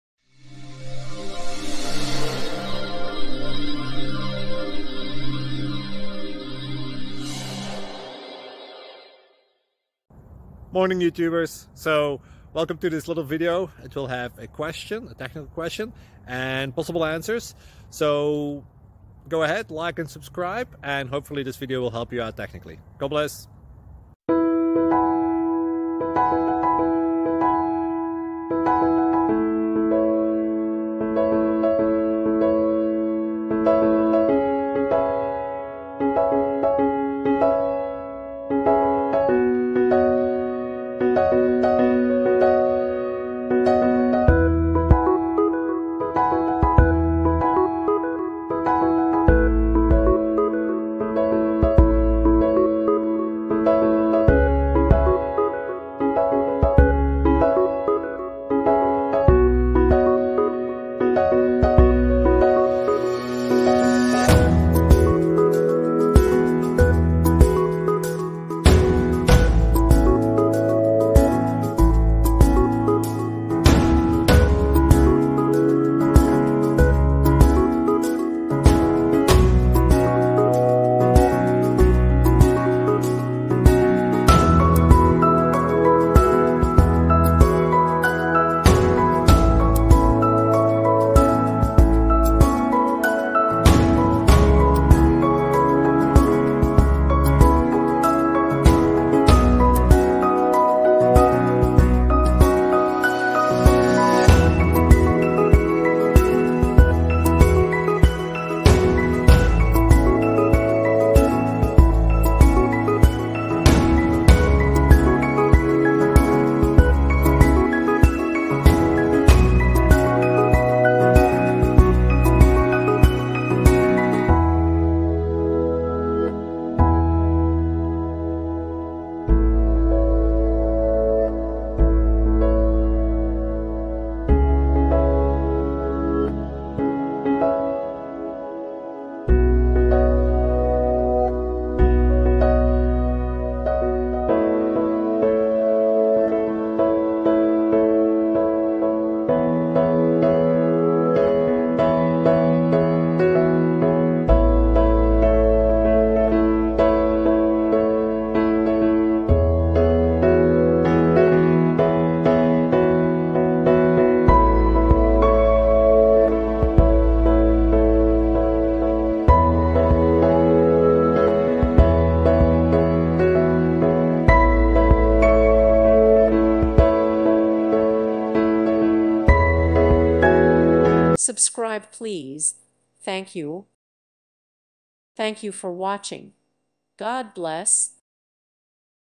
What are the deciding factors in choosing to expose a web service as a SOAP or REST service?
Software Engineering: What are the deciding factors in choosing to expose a web service as a SOAP or REST service?